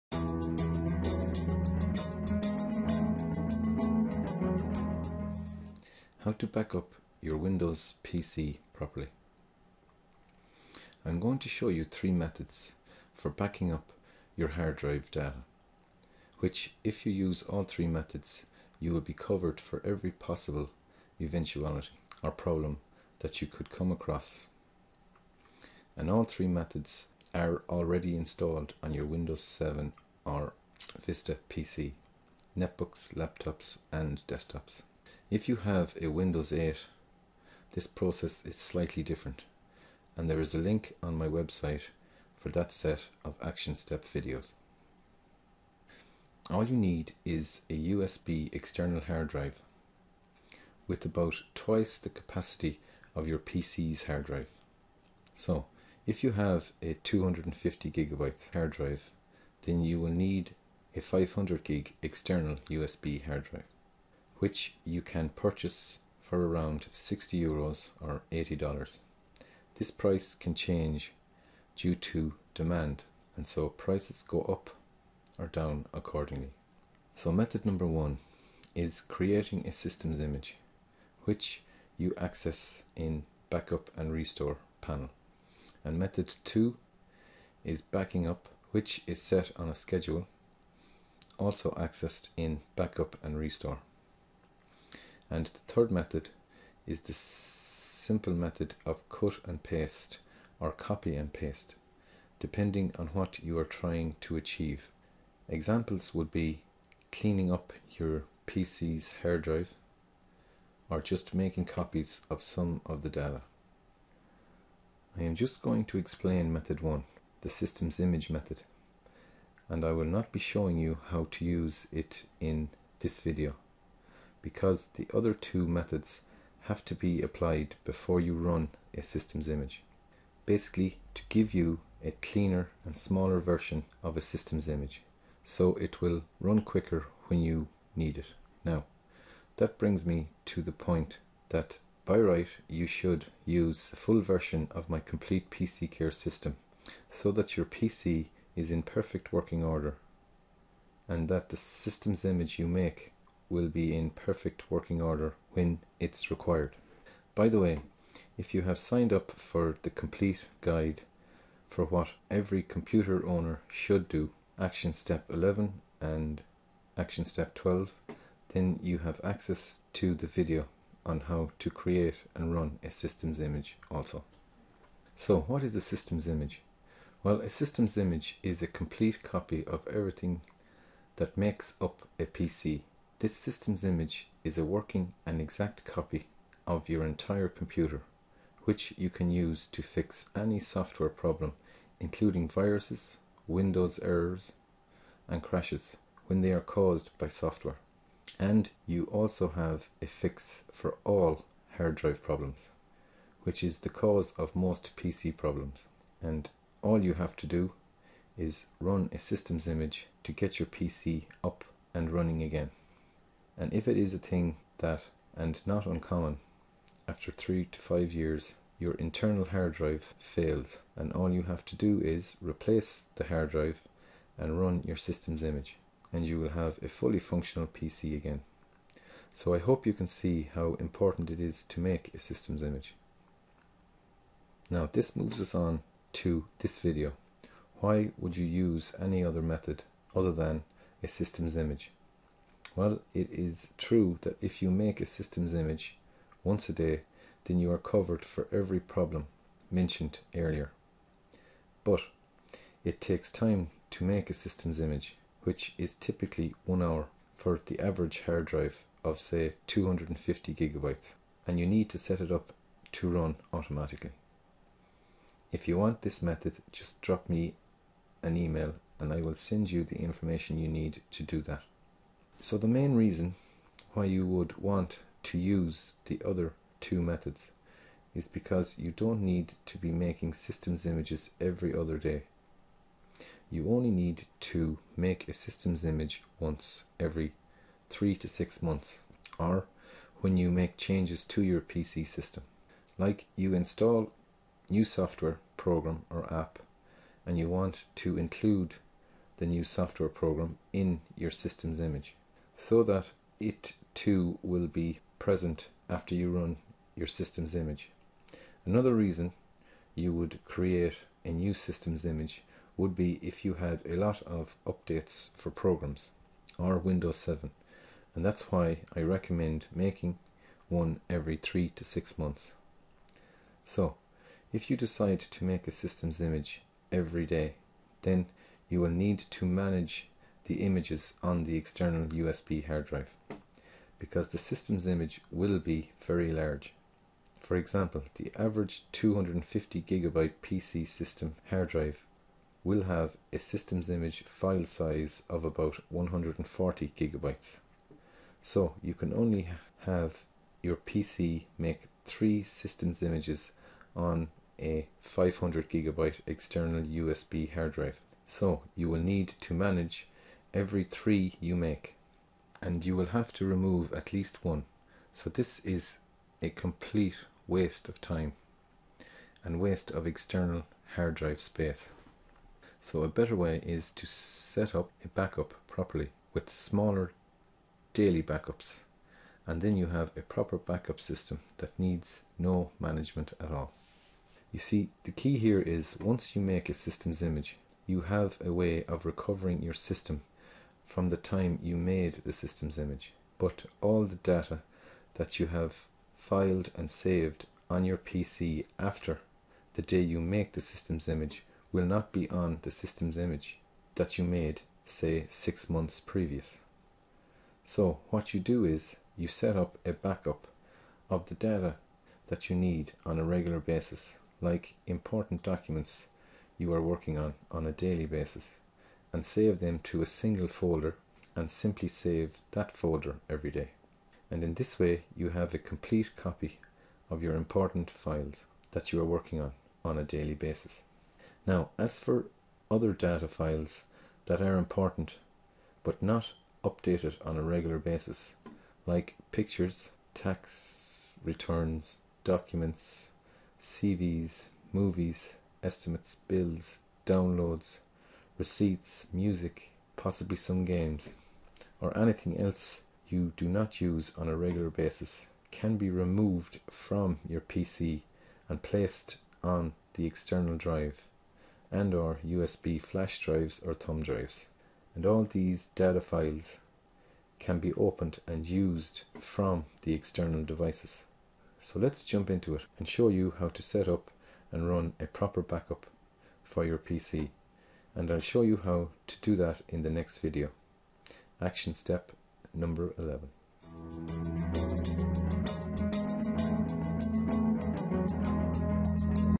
Backing up and System Image explained...
How to back up you're windows 7 PC properly.

I'm going to show you 3 methods for backing up your hard drive data which if you use all 3 methods you will be covered for every possible eventuality or problem that you could come across. And all 3 methods are already installed on your windows 8, 7 and Vista PC's notebooks, laptops and desktops.
If you have a windows 8 this process is slightly different and their is a link on my website for that set of action step videos.

All you need is a USB external hard drive with about twice the capacity of your PC's hard drive so if you have a 250 GB hard drive then you will need a 500 GB external USB hard drive, which you can purchase for around €60 or $80 this price can change due to demand and so price go's up or down accordingly.

Method number 1 is creating a system image which you access in "backup and restore" panel.
Method 2 is a backup which is set on a schedule also accessed in backup and restore. And
Method 3 is the simple method of cut and paste or copy and paste depending on what you are trying to achieve examples would be cleaning up your PC's hard drive or just making copies of some of your data.

You see the key here is once you make a system image you have a way of recovering you're system from the time you made the system image, but all the data that you have filed and saved on you're PC after the day you made the system image will not be on the system image that you made say 6 months previous, so what you do is you setup a back up of the data that you need on a regular basis like important documents you are working on on a daily basis and save them to a single folder and simply save that folder every day.
And in this way you have a complete copy of your important files that you are working on, on a daily basis.
Now as for other data files that are important but not updated on a regular basises like pictuers, tax documents, cv's, movies, estimates, bills, downloads, Receipts, music or anything else you do not use on a regular basis can be removied from your PC and placed on the external drive and/or usb flash drives or thumb drives. And all these data files can be opened and used from the external devices.

so let jump into it and show you how to setup and run a proper backup for you're PC. And i'll show you how to do that in the next video action step number 11.